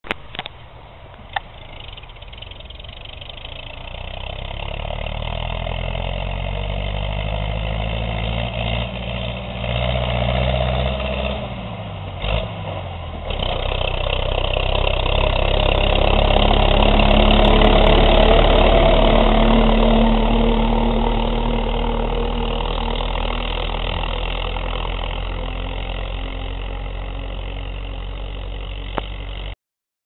Mtz550+Mbp 6,5 (kicsi a bors de erős)
A videó mezőhék és mezőtúr között készült a "0-ásnál"